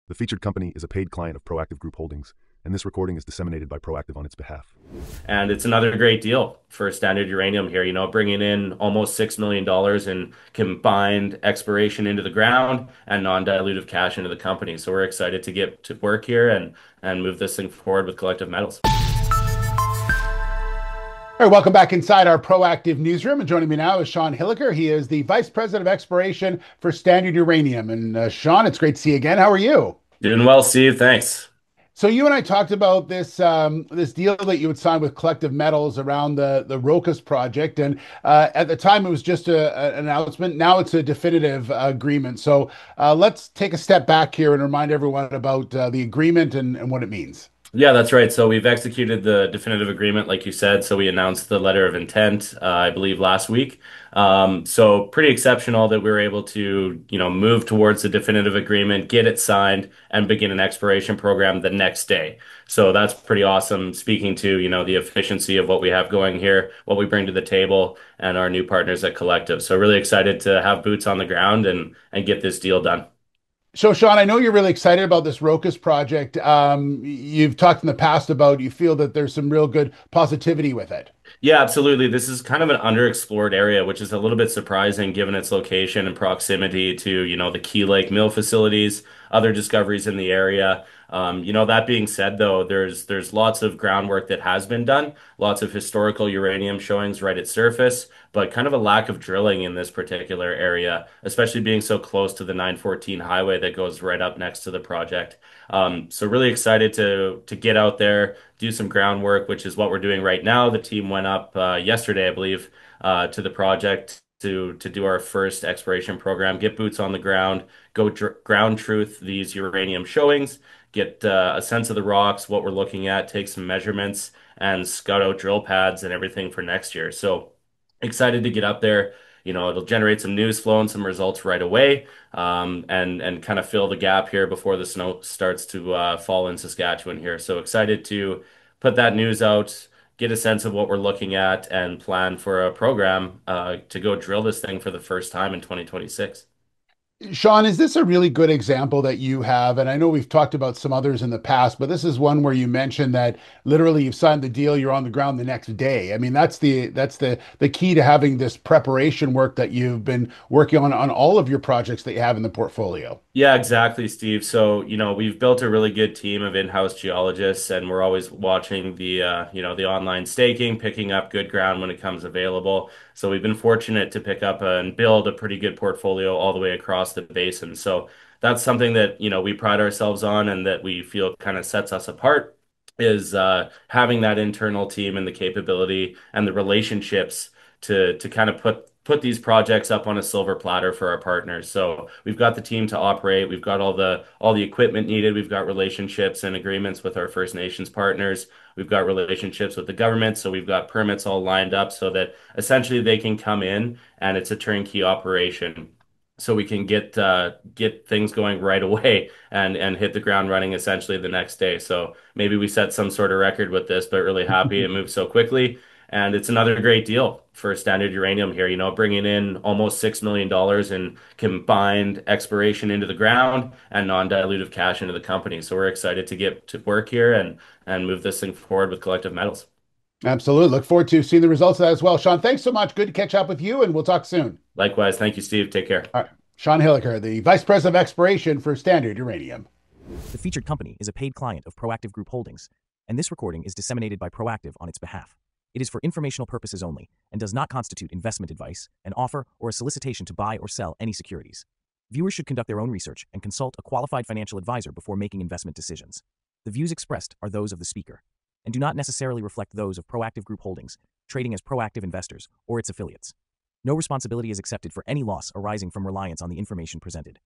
Standard Uranium finalizes 75% earn-In deal with Collective Metals at Rocas project
Standard Uranium Vice President of Exploration Sean Hillacre joined Steve Darling from Proactive to announce a key milestone for the company. The previously signed letter of intent with Collective Metals Inc.—which outlined a pathway for Collective to acquire a 75% interest in the Rocas Project through a three-year earn-in option—has now advanced to a definitive property option agreement.

The Rocas Project spans 4,002 hectares in the highly prospective eastern Athabasca Basin region of northern Saskatchewan, an area renowned for hosting world-class uranium deposits.

Strategically located approximately 75 kilometres southwest of Cameco’s Key Lake Mine and Mill complex and 72 kilometres south of the current Athabasca Basin margin, Rocas holds a prime position in one of the world’s most prolific uranium districts.

Initially staked by Standard Uranium in May 2023, the project was recently expanded by an additional 931 hectares, further consolidating the company’s footprint across this underexplored corridor. Hillacre emphasized that the expanded land package significantly enhances the exploration potential, particularly given its structural and geological setting.

As part of its technical work, Standard Uranium recently completed a high-resolution ground gravity survey along key conductive exploration trends at Rocas. These surveys provided valuable subsurface insights, helping to refine the company’s geological understanding and sharpen the definition of future high-priority drill targets.

Hillacre also confirmed that the company’s technical team is now on-site at Rocas, conducting detailed mapping, prospecting, and sampling programs. The goal is to ground-truth historical uranium showings observed at surface and to advance the project toward drilling readiness. Grab samples collected during this phase will be sent to the Saskatchewan Research Council Geoanalytical Laboratories in Saskatoon for comprehensive geochemical analysis.

With the agreement finalized and fieldwork underway, Standard Uranium is positioning the Rocas Project as a high-potential discovery opportunity within its growing portfolio of Athabasca Basin uranium assets.